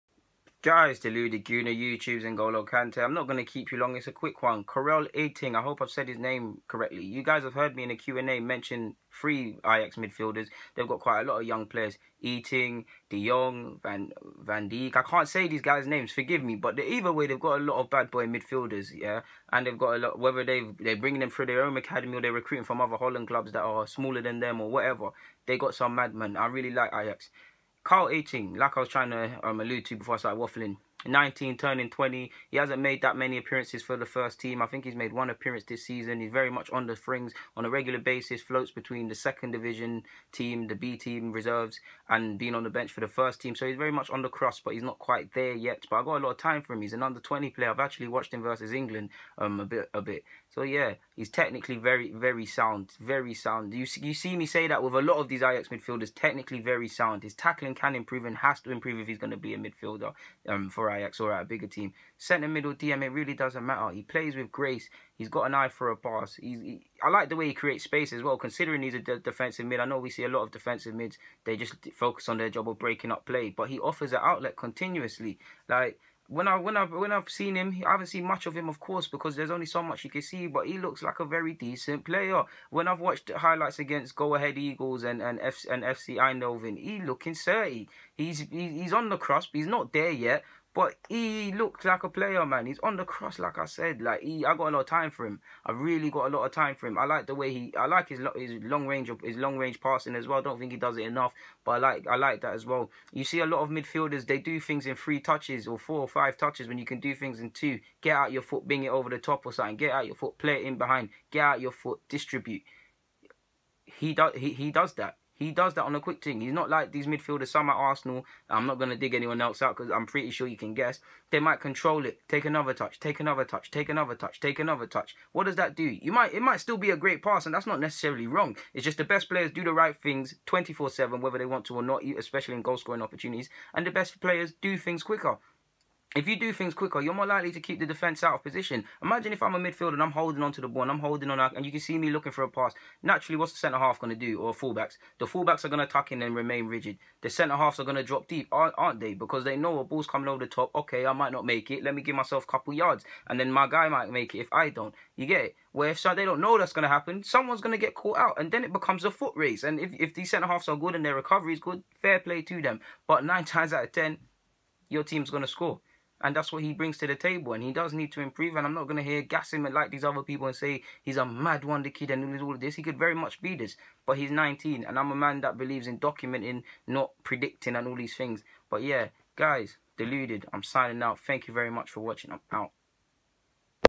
Arsenal FC : Carel Eiting Cold be a star in our midfield !!
19  YEARS OF AGE PLAYING FOR AJAX LET US TALK ABOUT HIM.